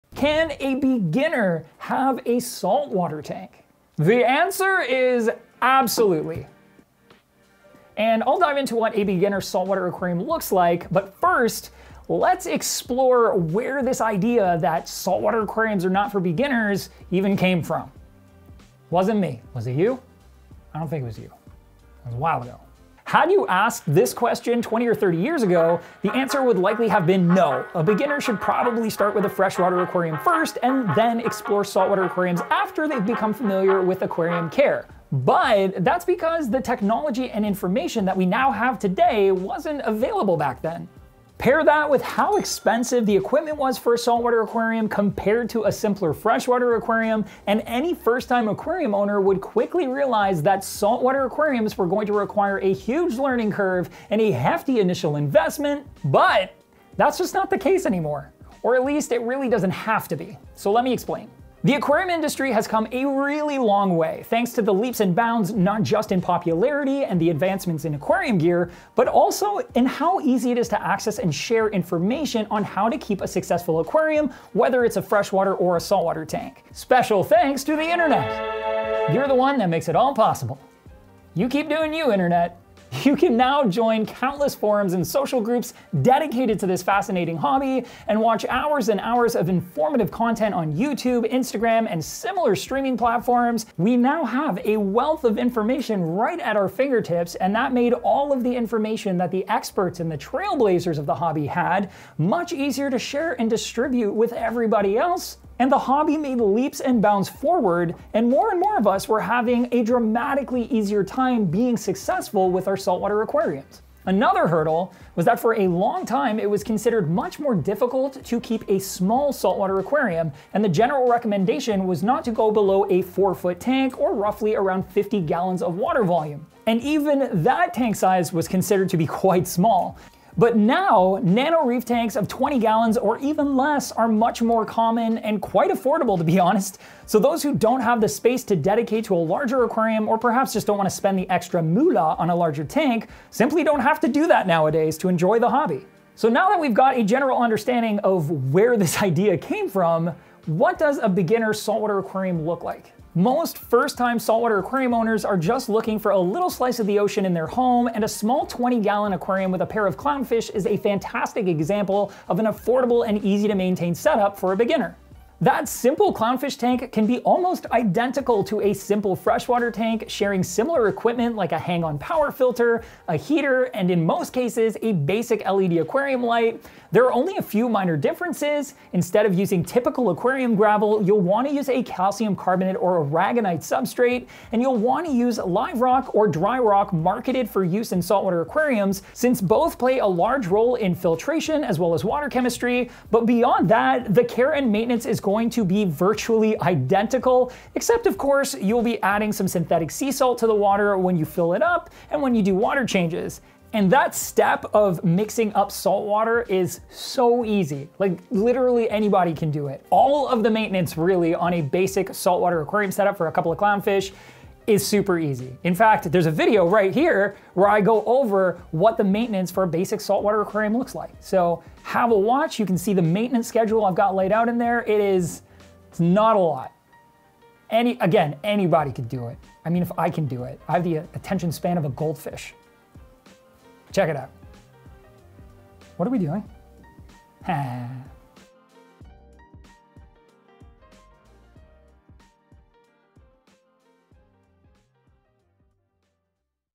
Can a Beginner Have a Saltwater Tank?
Can a beginner jump right into keeping saltwater aquariums? Yes! Keeping a saltwater aquarium has never been easier for beginners, and Thomas is here to break it all down for us. He'll be covering where to get the best information as well what a standard beginner aquarium looks like, so, be sure to check it all out here!